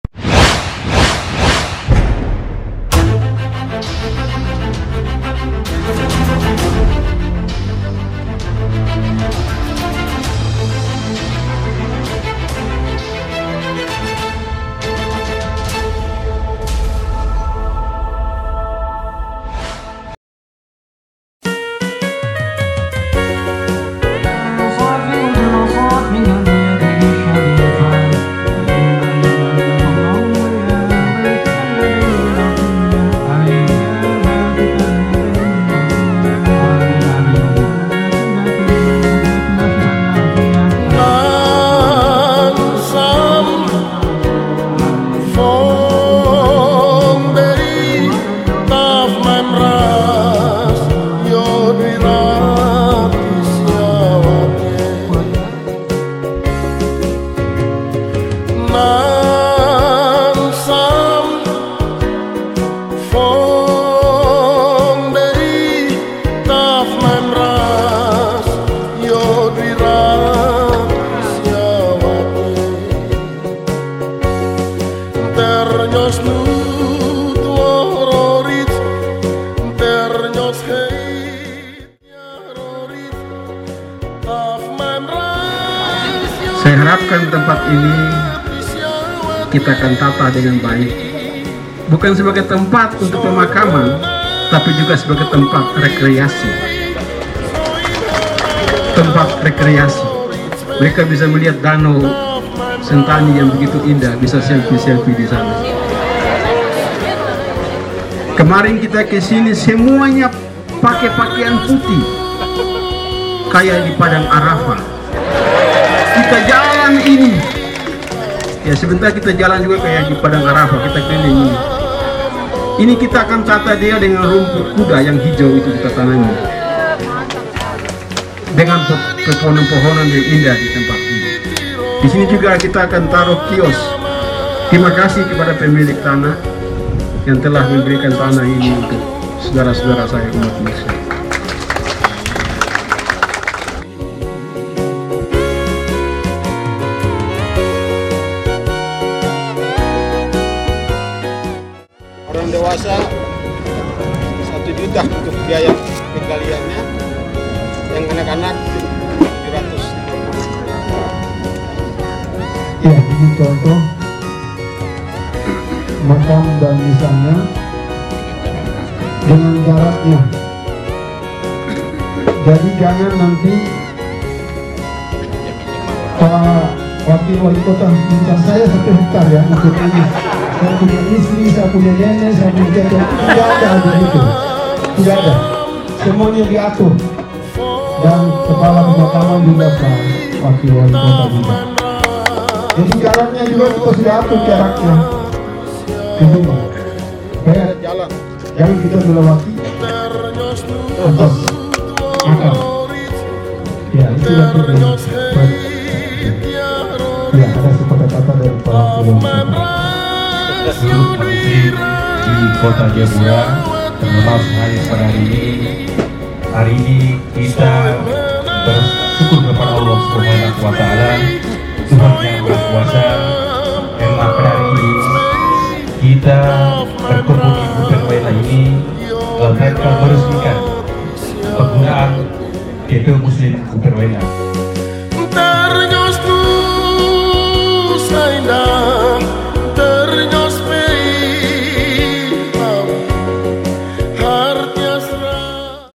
PERESMIAN TPU MUSLIM BUPER
Ratusan Umat Muslim utusan Masjid dan Mushola se-kota jayapura menghadiri syukuran pengoperasian lokasi pemakaman Umum Muslim di Buper distrik heram.